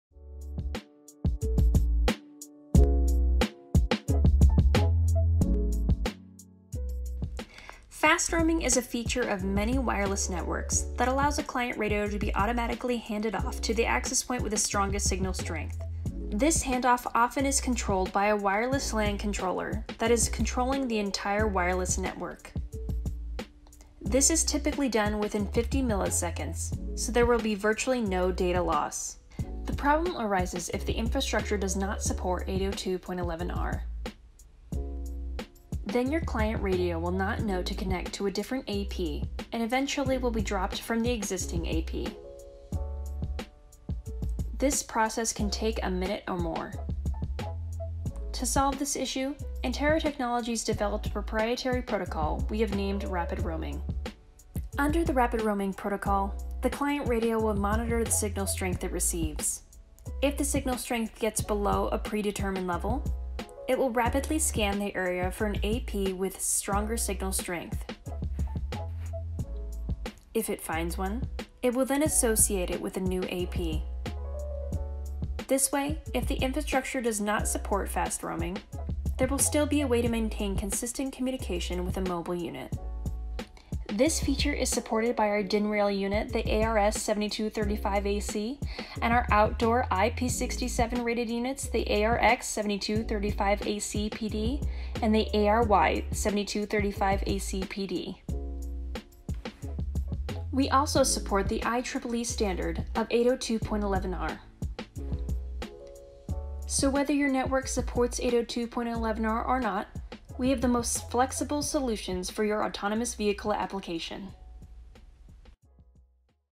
Antaira's New Wireless Feature: Rapid Roaming
Antaira Technologies is excited to announce a new 802.11ac Wireless router and Access Point (AP) feature called Rapid Roaming. This is an alternative option for applications where the infrastructure does not support 802.11r Fast Roaming.